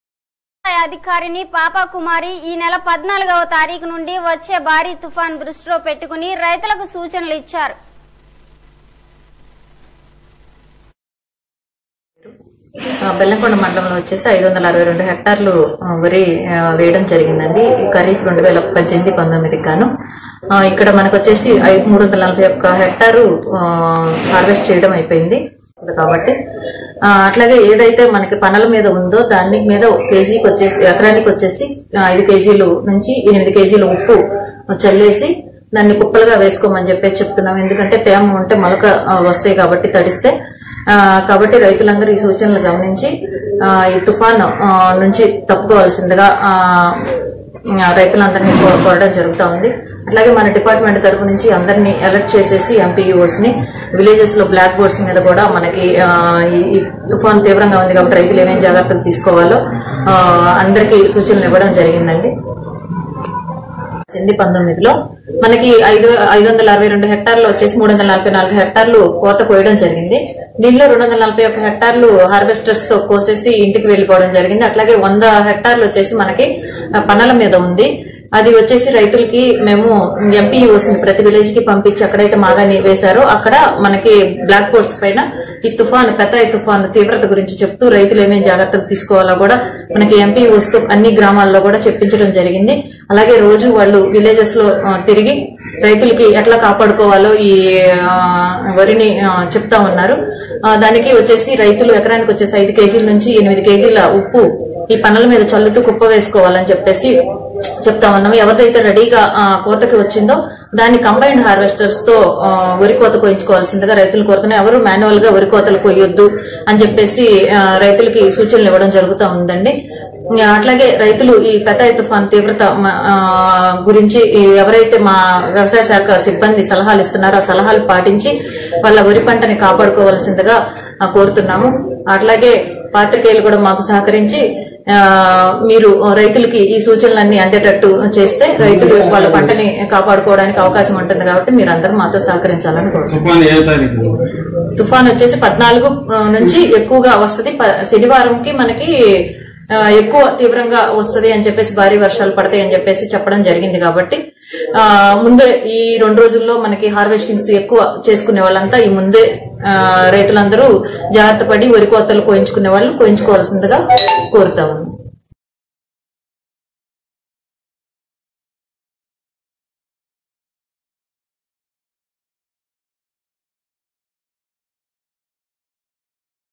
గుంటూరుజిల్లా: బెల్లంకొండ మండలం వ్యవసాయ
గుంటూరుజిల్లా: బెల్లంకొండ మండలం  వ్యవసాయ అధికారిని  పాప కుమారి ఈ నెల 14 వ తారీకు నుండి వచ్చే భారీ తుఫాను దృష్టిలో పెట్టుకుని రైతులకు సూచనలు ఇచ్చారు